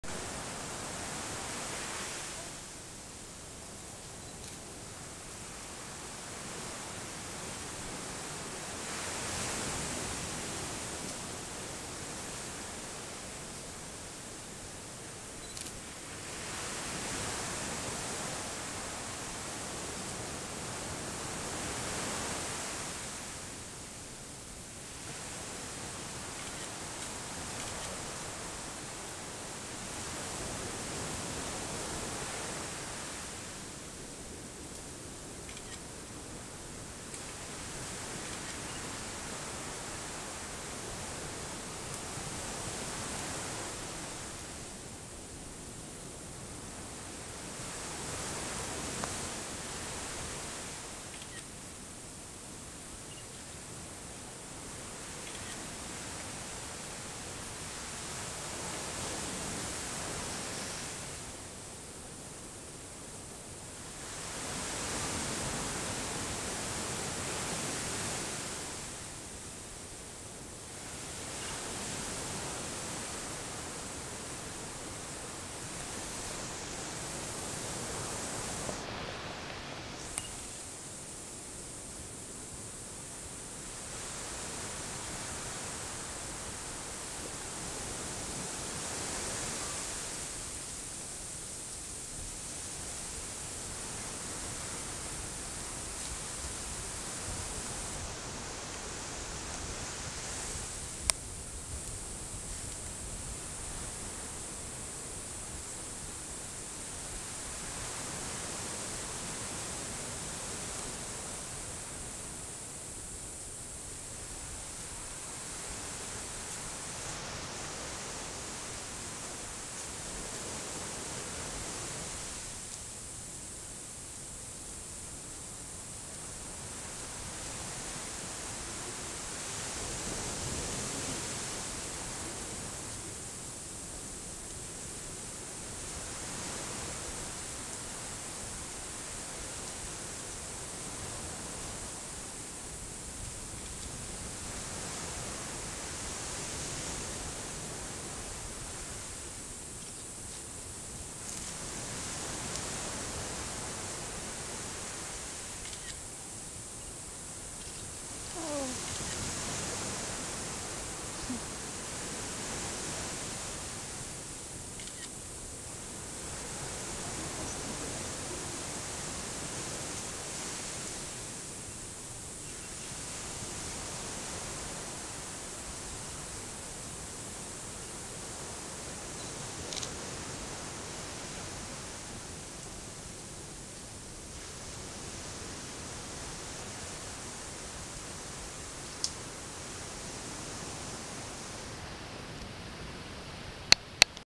Mama and baby sloth
So lucky. Not only does this sloth have a baby, but she's coming down to go to the bathroom -- which only happens once a week. Otherwise, they'd be way high in the treetops. In Cahuita National Park, Costa Rica... ocean waves in background.